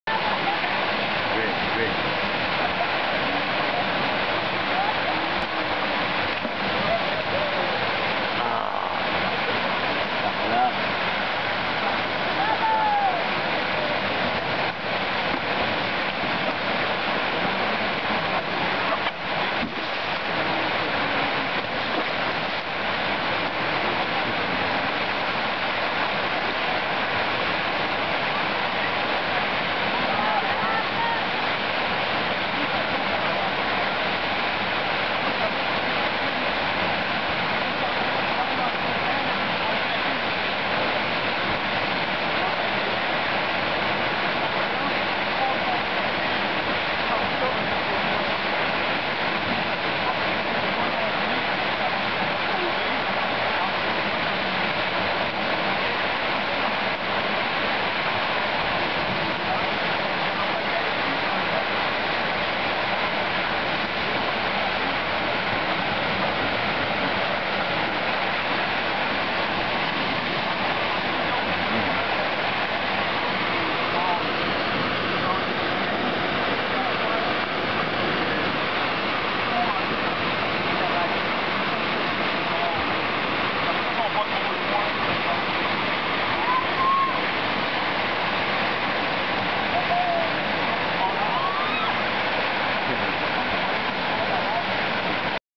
Surf à Montréal - la Vague à Guy sept 2012
Vidéo du mois: septembre 2012
Filmé dans l'arrondissement de LaSalle, Montréal